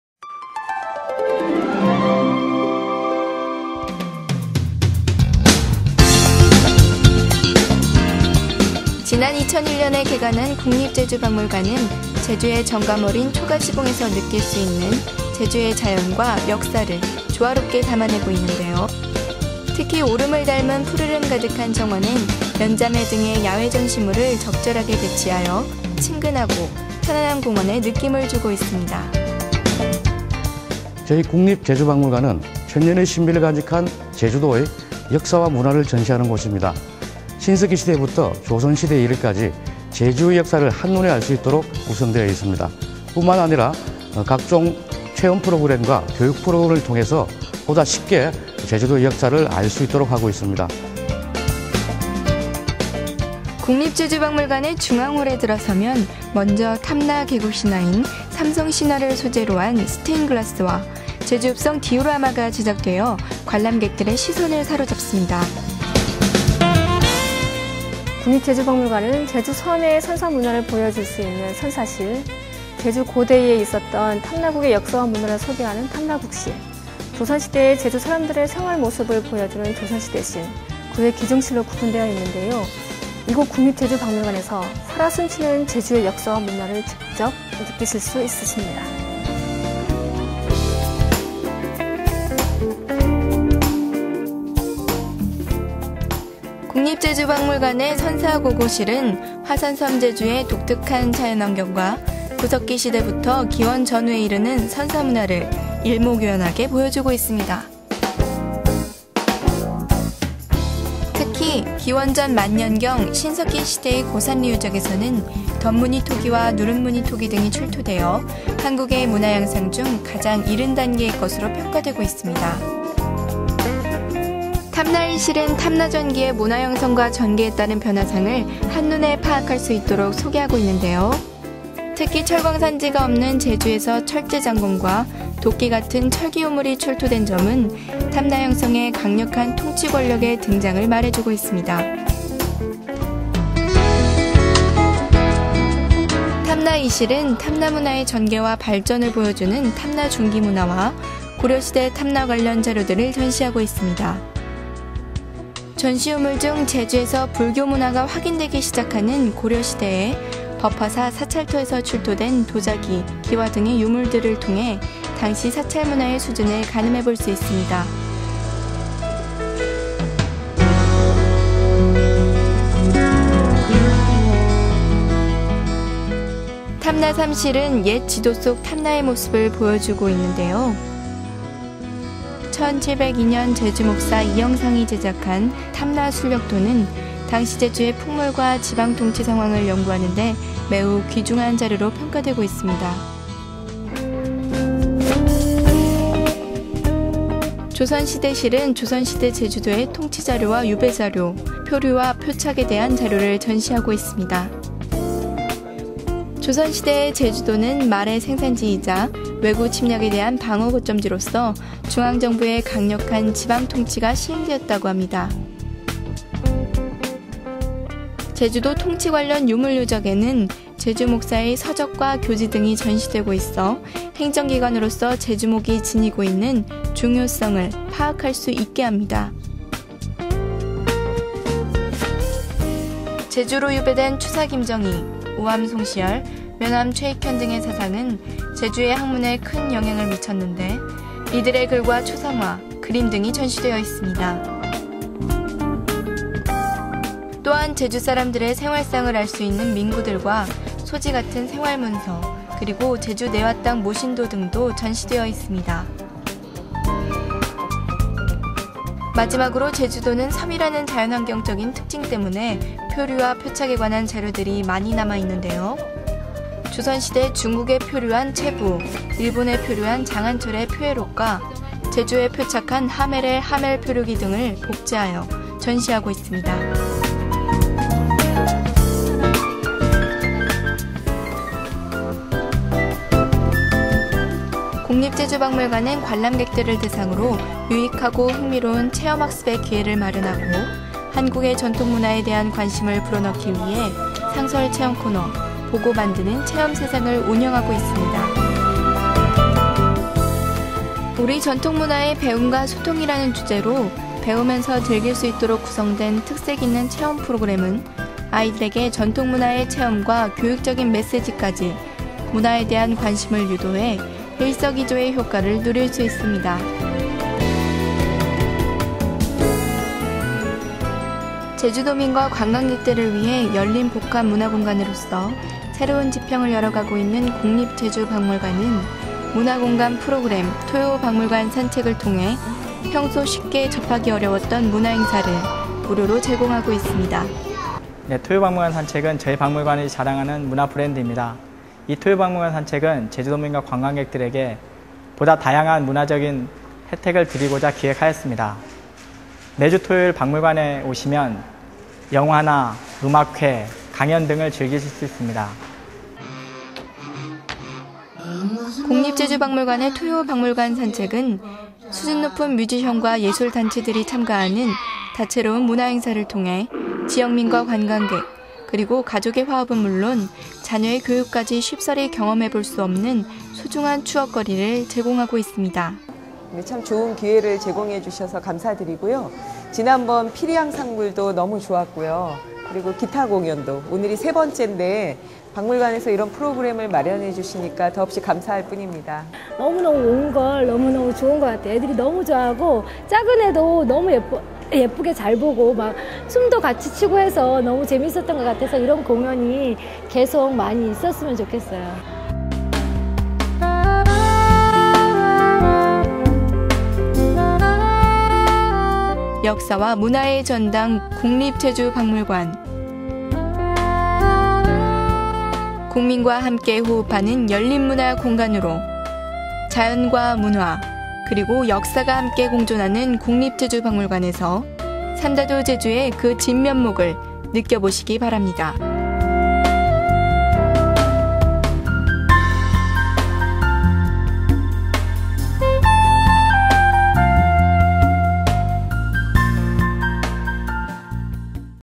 Jeju National Museum_국립제주박물관
Jeju, which boasts its superb natural landscape.
It is the Jeju National Museum, where the history and culture of Jeju are still alive.
The Jeju National Museum opened in 2001, and its thatched roof well represents the nature and history of Jeju.
Green garden is well decorated with displays, which gives tourists comfort and familiarity.
There is a thing that attracts your attention when you enter into the hall of the museum.
It is the diorama, which stipulates the myth of Jeju and its architectures and nature.
Jejueupseong Fortress perfectly re-presents the fortress that was depicted in Tamna sullyeokdo (Paintings of the Inspection Tour of Jejudo Island) in 1702.
The Prehistory Gallery displays the unique natural environment of Jeju, as well as a prehistoric story of the ancestors and their culture.
Tamna Period Gallery I displays the history from B.C to the period of the three kingdoms, showing the change in culture, represented by changes in earthenware.
Especially, a discovery of steel swords and axes in the island, with an absence of steel-mines show that Jeju had once been colonized by a stronger country.
Tamna Period Gallery 2 presents the progress and development of Mid-Tamna culture, displaying relics and information.
Mid-Tamna period is the era of the latter period of the three kingdoms. It was the period when Jeju, in name of Tamna developed rapidly in terms of trade and interaction with the foreign countries.
Tamna Period Gallery 3 shows Tamna in ancient maps.
Tamna sullyeokdo drawn up in 1702 by Lee Hyeong-sang, is valued highly in the study of the politics of historical Jeju.
Joseon Period Gallery displays the political data, information on lifestyle, and the history of drifting ashore.
Jeju functioned as a horse farm, a place of exile, and a defense base against foreign invasions.
The Jeju National Museum provides an event, called 'Experience World' to the people to help them learn and experience about the traditions and culture of Korea.
In the theme of 'Learning and Communication', children are invited to learn and experience the traditional culture of Korea and to boost their interest and understanding.
Moreover, with the opening of the museum for extended hours every Saturday, the museum offers people various cultural activities and events free of charge.
Many musicians and art groups come every Saturday to promote harmony between family members and to maximize the education of the children.
The center for beautiful history and culture, the Jeju National Museum.
Come to the Jeju National Museum, a center for the development of Jeju culture, open cultural space and a place where history, culture and nature co-exist.